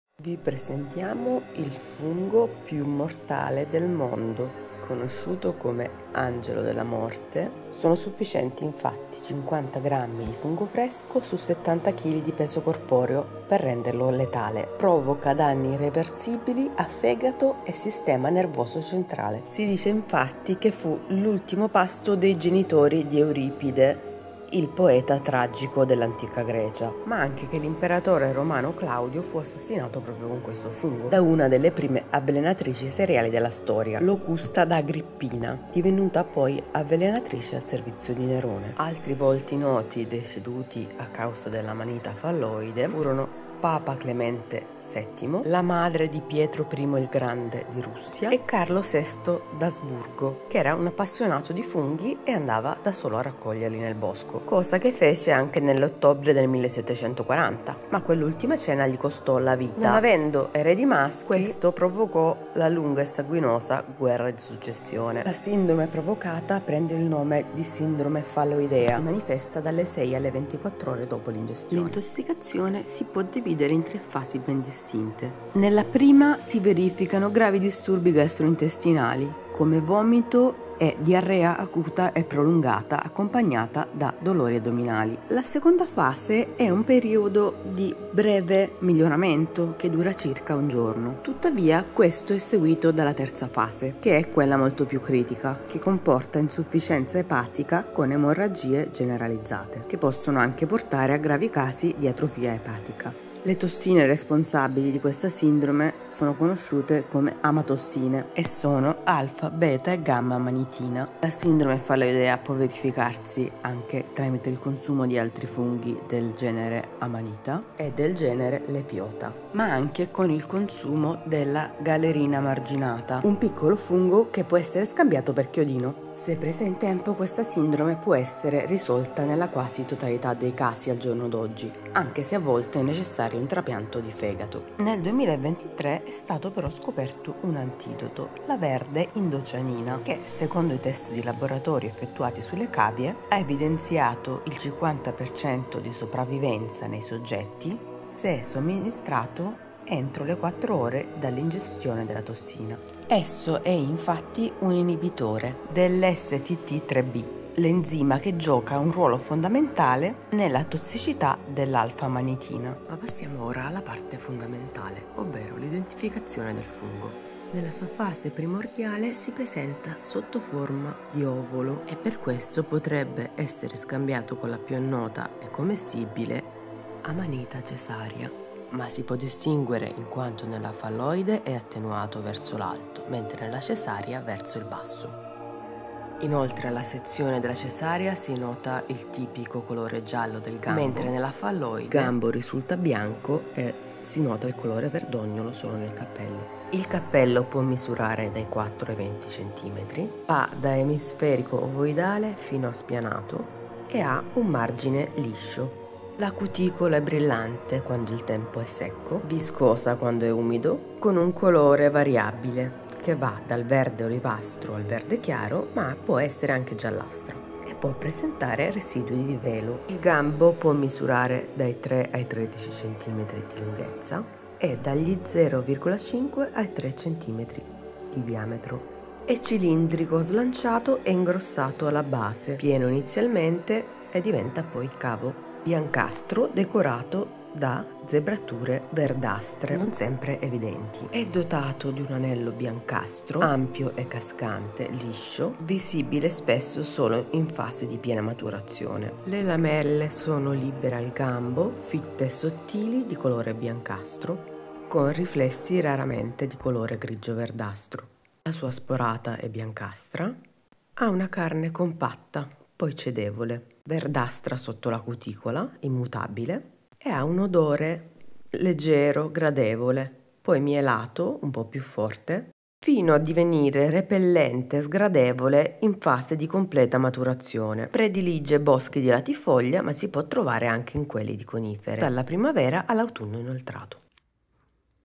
Il fungo più mortale ☠️al mondo: 🍄Amanita phalloides. Scopri come riconoscerlo e salvarti la vita!
Amanita phalloides🍄: il fungo più mortale al mondo, un killer ☠️  silenzioso nei nostri boschi.

Conosciuta anche  come Ovolo bastardo, Angelo della morte, Tignola verdognola questa specie è tra le più velenose al mondo! É spesso confusa con l'Amanita caesarea, l'ovolo buono.

☝Ricorda che in Italia è vietata la raccolta di ovoli chiusi di Amanita caesarea!

⚠️ La sua tossicità può essere fatale!

Approfondisci le caratteristiche morfologiche , l'habitat e la tossicità di uno dei funghi più temuti dai micologi e come il suo veleno ha determinato le sorti di popoli e nazioni.

🍄Impara a riconoscere le sottili differenze che la distinguono dai funghi commestibili.

Video indispensabile per chi ama raccogliere funghi.

bibliografia:

Nicolò Oppicelli, Funghi in Italia, edizione 2024

Wang B, Wan AH, Xu Y, Zhang RX, Zhao BC, Zhao XY, Shi YC, Zhang X, Xue Y, Luo Y, Deng Y, Neely GG, Wan G, Wang QP. Identification of indocyanine green as a STT3B inhibitor against mushroom α-amanitin cytotoxicity. Nat Commun. 2023 May 16;14(1):2241. doi: 10.1038/s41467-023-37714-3. PMID: 37193694; PMCID: PMC10188588.